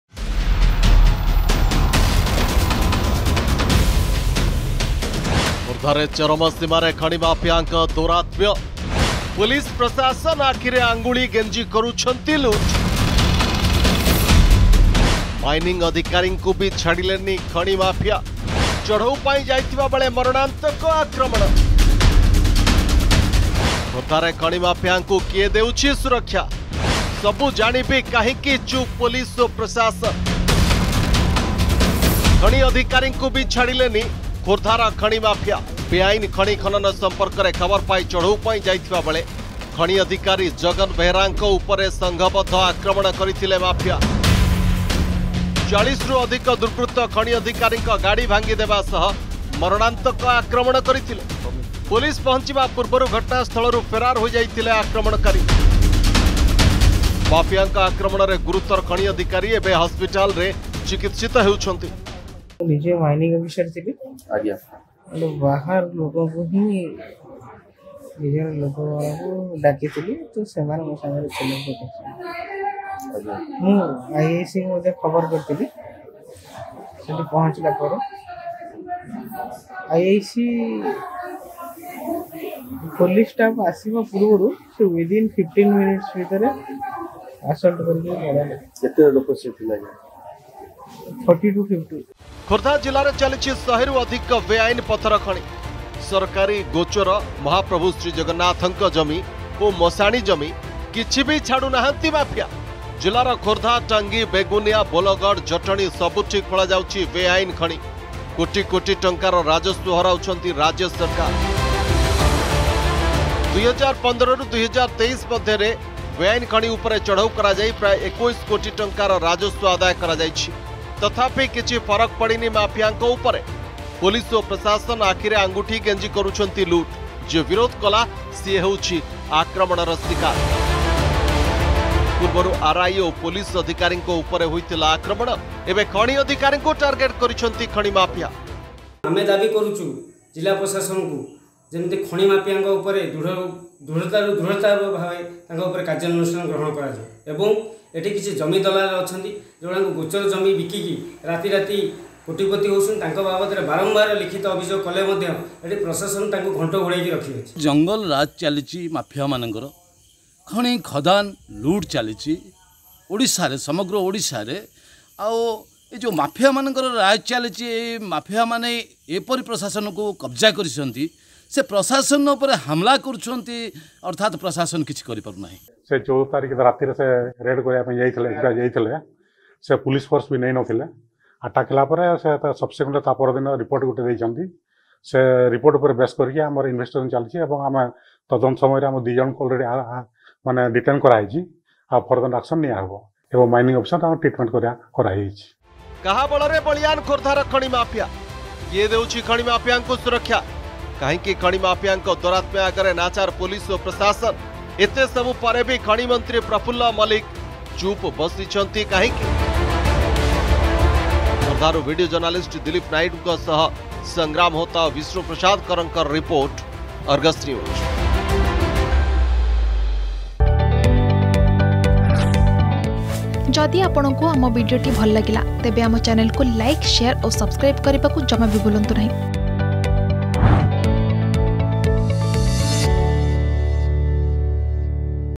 ଖୋର୍ଦ୍ଧାରେ ଚରମ ସୀମାରେ ଖଣି ମାଫିଆଙ୍କ ଦୌରାତ୍ମ୍ୟ || Mining Loot || Khordha
ଖୋର୍ଦ୍ଧାରେ ଚରମ ସୀମାରେ ଖଣି ମାଫିଆଙ୍କ ଦୌରାତ୍ମ୍ୟ । ଆଇନ ଆଖିରେ ଆଙ୍ଗୁଳି ଗେଞ୍ଜି କରୁଛନ୍ତି ଲୁଟ୍ । ଖଣି  ଅଧିକାରୀଙ୍କୁ କଲେ ମରଣାନ୍ତକ ଆକ୍ରମଣ । ଖୋର୍ଦ୍ଧାରେ ଖଣି ମାଫିଆଙ୍କୁ କିଏ ଦେଉଛି ସୁରକ୍ଷା ।